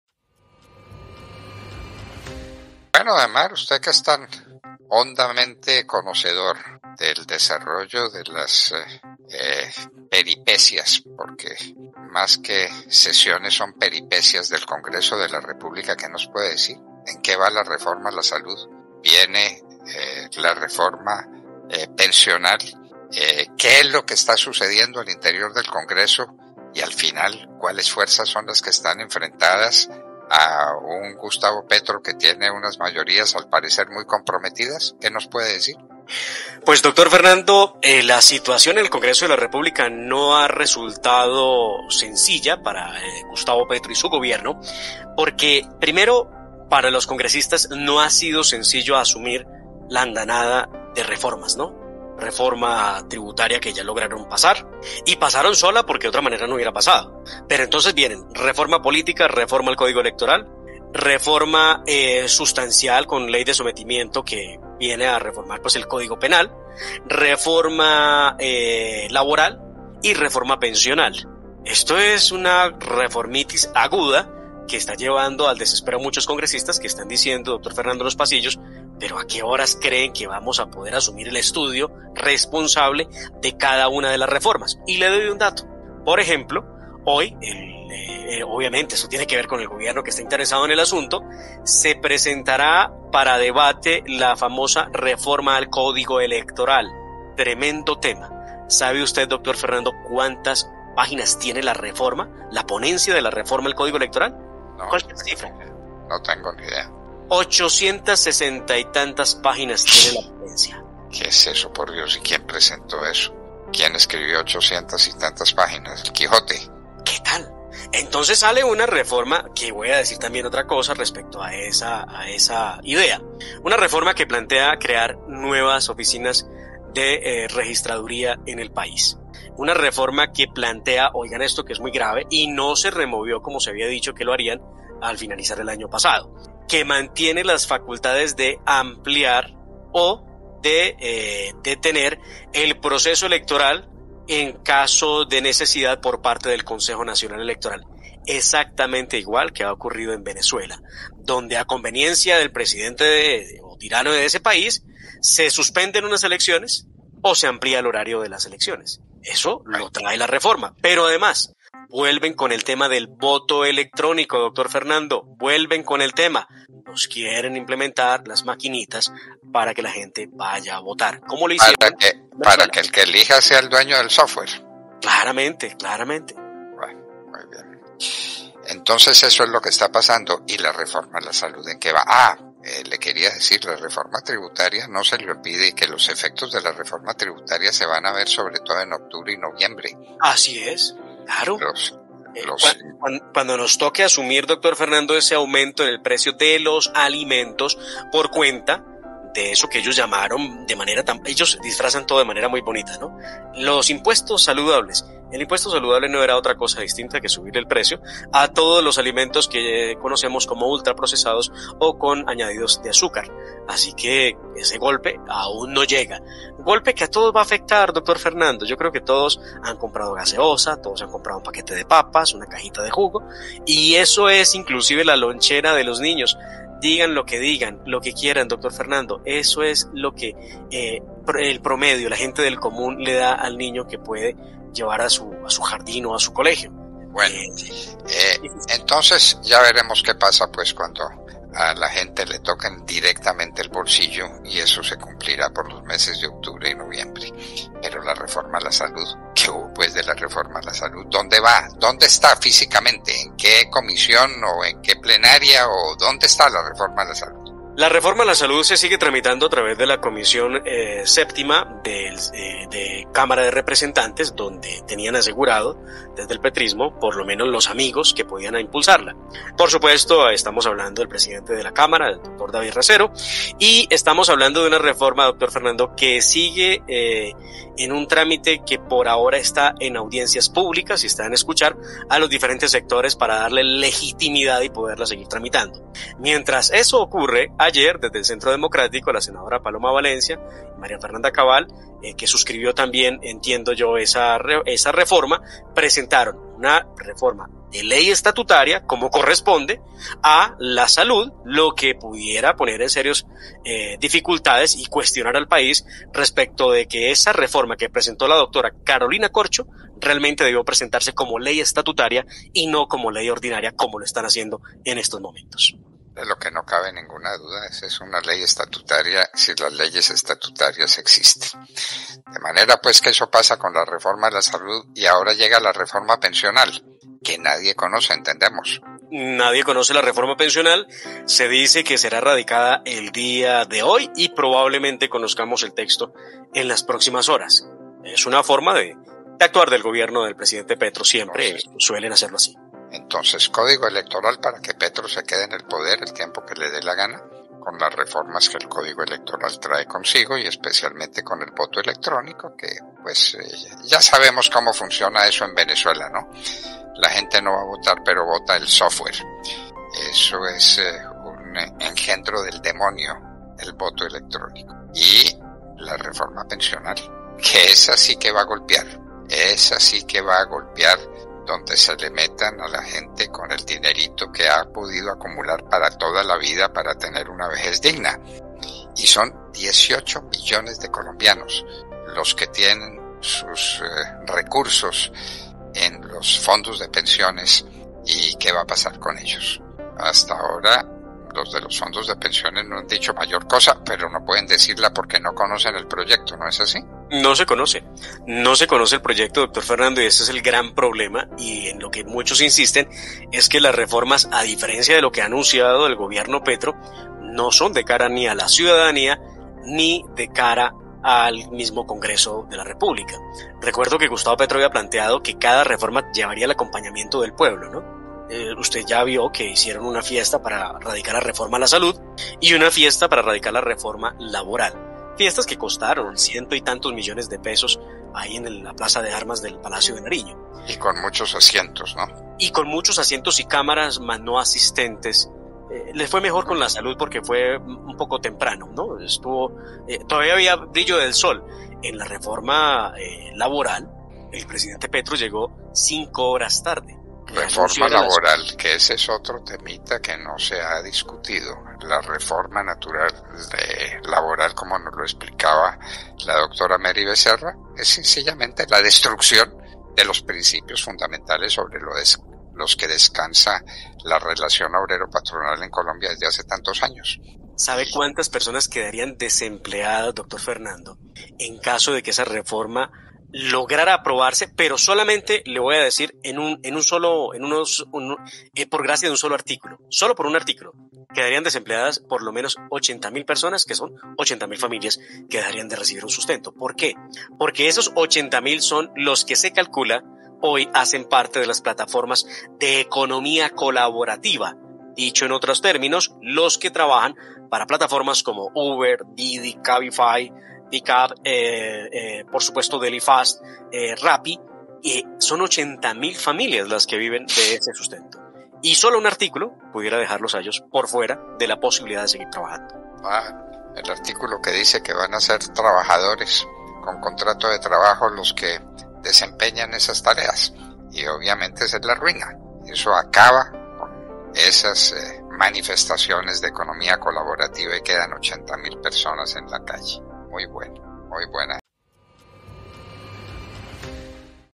"REFORMITIS AGUDA" 🚨 NO LA TIENE FÁCIL EL GOBIERNO PETRO
Reforma al código electoral, el nuevo golpe que podría recibir el Gobierno